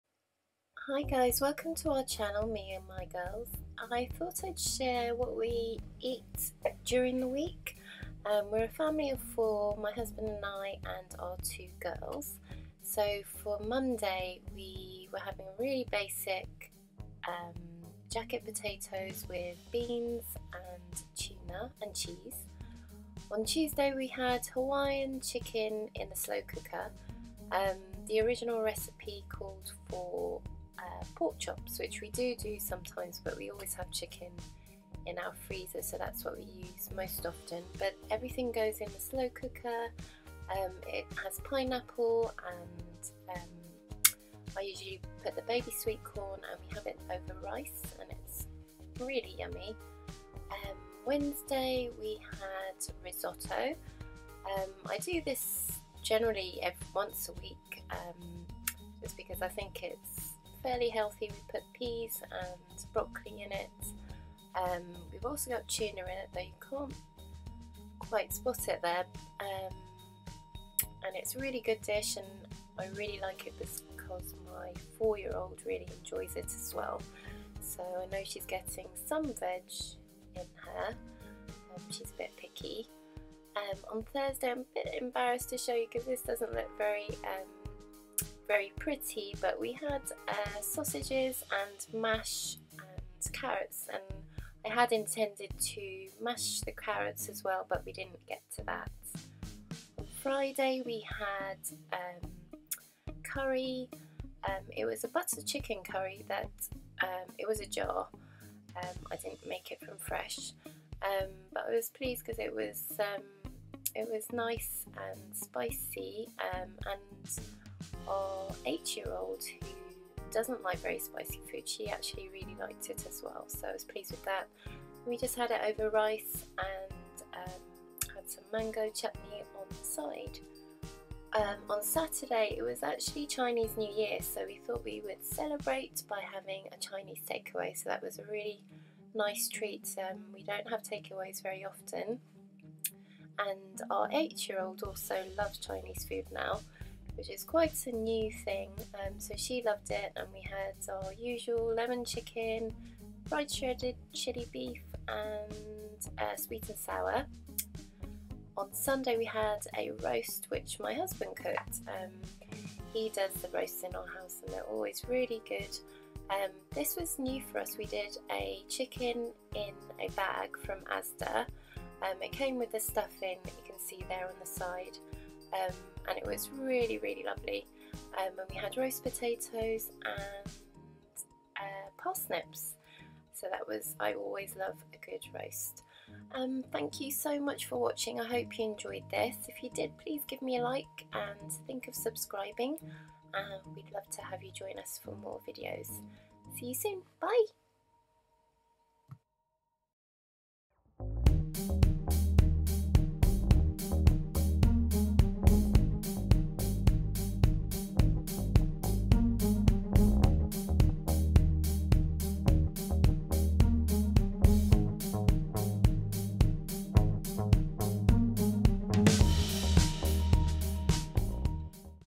Meals of the Week | Evening Meals
Meals of the Week for Our Family of Four | January 2020

Hello! Welcome to Me and My Girls! I thought I would share our evening meals for a week. I love seeing what other people are eating and this year I’m trying to plan our meals a little better so I thought this would be a fun video to put together. Hope you enjoy it!